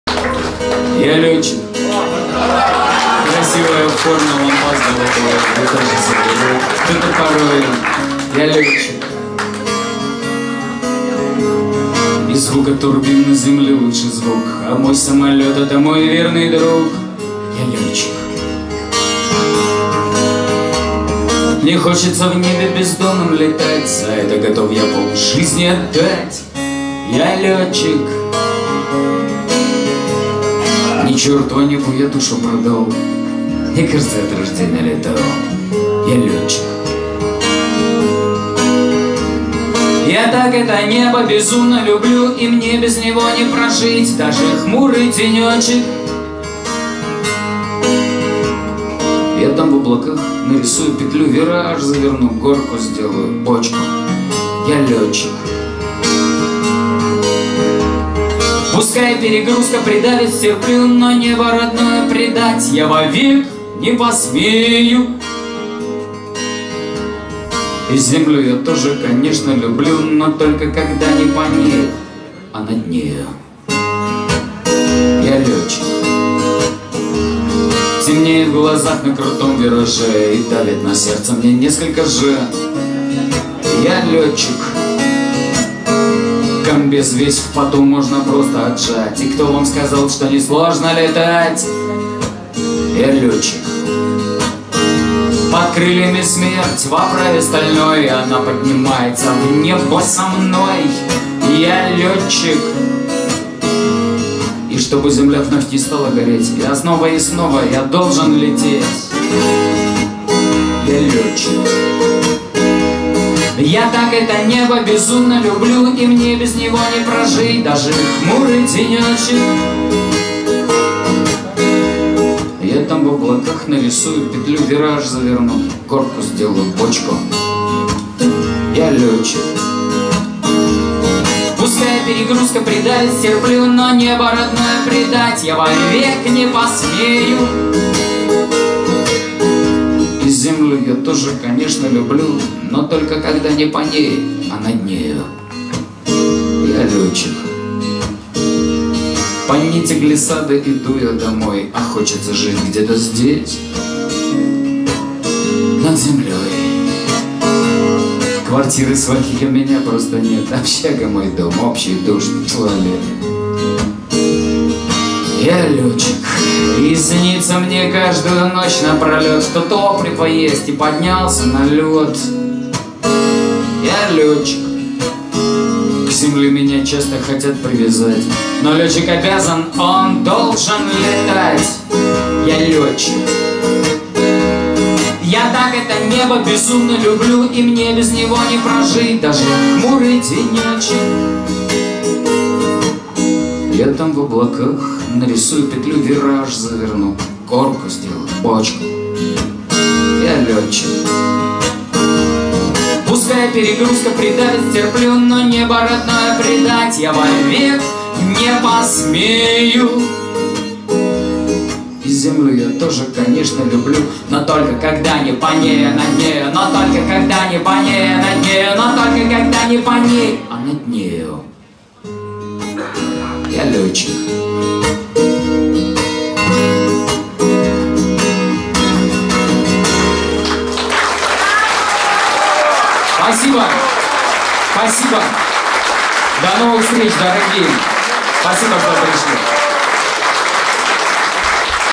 Н.Анисимов Гнездо глухаря 10.11..2017 - финал.
Автор-исполнитель, военный тележурналист,  подполковник запаса Николай АНИСИМОВ выступил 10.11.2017 г. в Гнезде глухаря с авторской программой «Про самолёты, вертолёты и людей…»

Николай Анисимов, авиация, ВВС, ВКС,